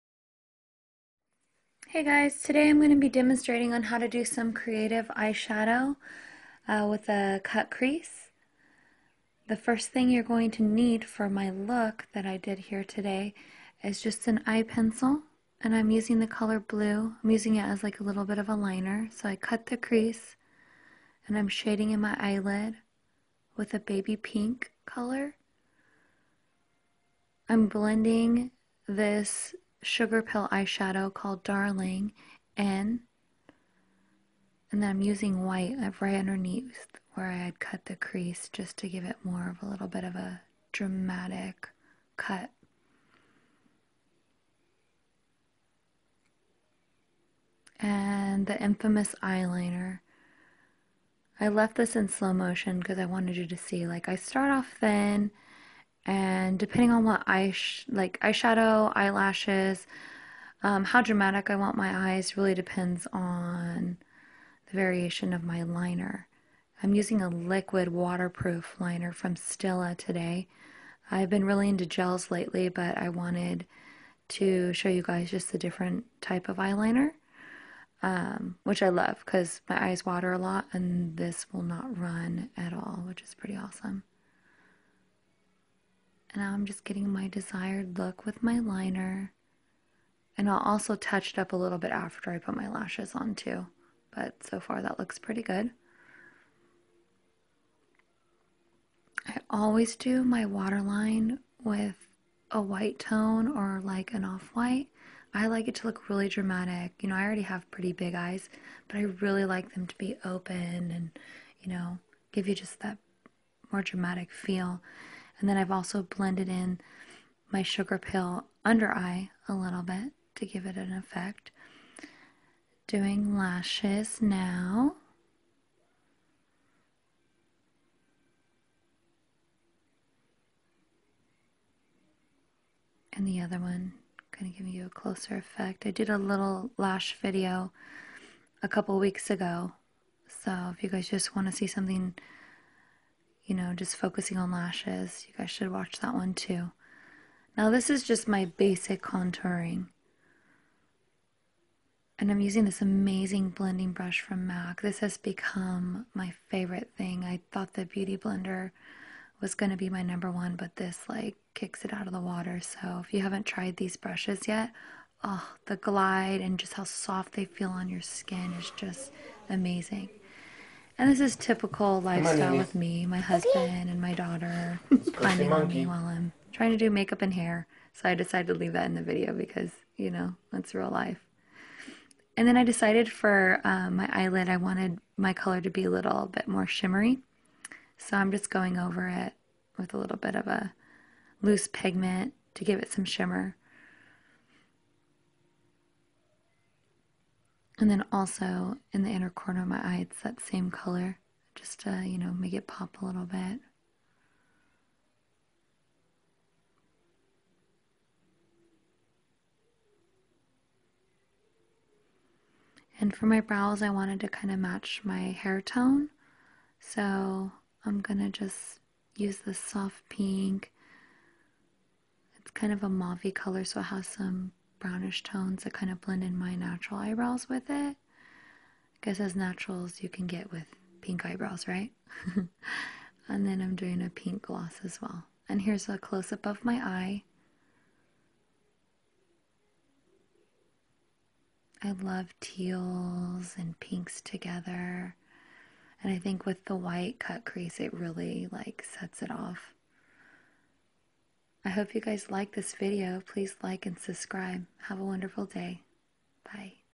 Eye shadow technique!
A typical day with me doing makeup! Watch me add some fun tones to my look!